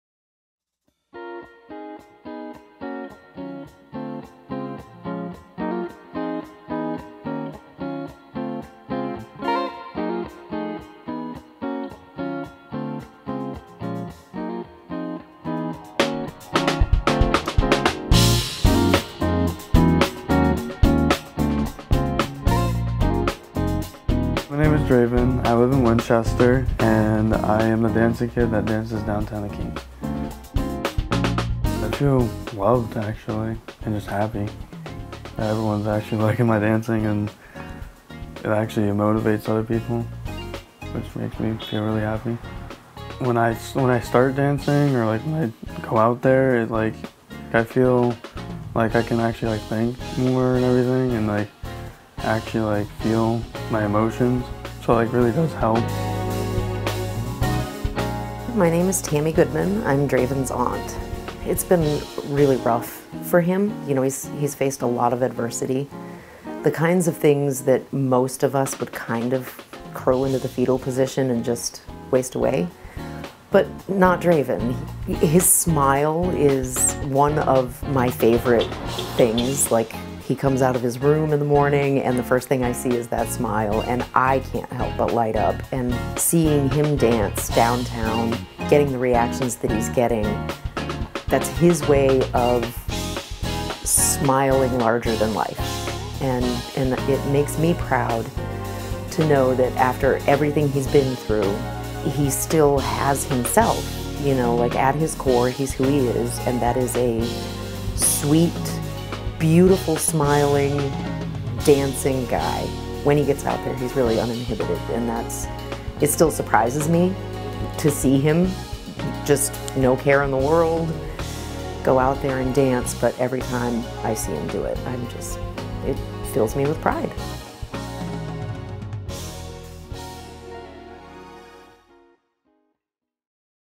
Dreyvin "The Dancing Kid" Hoskison
Some community members have called him “the guy who jams out” or “the dancing kid.” In reality, his name is Dreyvin Hoskison. And despite what some may think, the dancing is really just for fun, according to the 19-year-old Winchester resident.